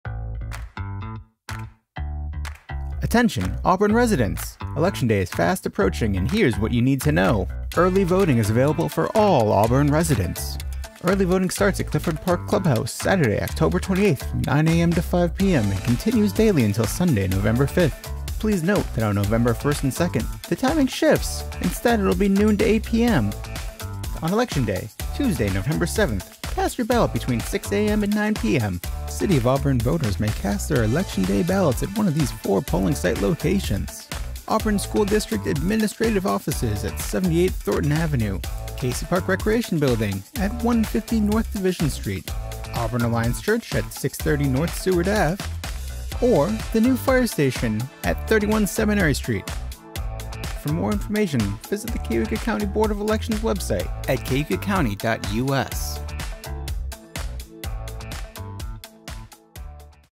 City of Auburn, NY - Early Voting & Election Day, Nov. 7, 2023 - Public Service Announcement
Election Day is November 7, 2023 and the Auburn Regional Media Access Channel(ARMA) has created a video PSA to assist voters with early voting and Election Day polling site information within the municipality of the City of Auburn, NY.

If you are a voter residing in the City of Auburn, NY here is the Election Day 2023 Poll information you will need to know:

EARLY VOTING IN THE CITY OF AUBURN - The Early Voting Poll Site is the Clifford Park Clubhouse (aka Y Field), 81 Mary Street, Auburn, NY  13021.

Early Voting Poll Site Schedule is as follows:

Saturday, October 28, 9 am – 5 pm
Sunday, October 29, 9 am – 5 pm
Monday, October 30, 9 am – 5 pm
Tuesday, October 31, 9 am – 5 pm
Wednesday, November 1, 12 pm (Noon) – 8 pm
Thursday, November 2, 12 pm (Noon) – 8 pm
Friday, November 3, 9 am – 5 pm
Saturday, November 4, 9 am – 5 pm
Sunday, November 5, 9 am – 5 pm

ELECTION DAY – TUESDAY, NOVEMBER 7, 2023 - Election Day Poll Sites in the City of Auburn will be open 6 a.m. through 9 p.m. on Tuesday, November 7, 2023.  You can cast your ballot at any of these 4 poll site locations.

For complete voter information visit the Cayuga County Board of Elections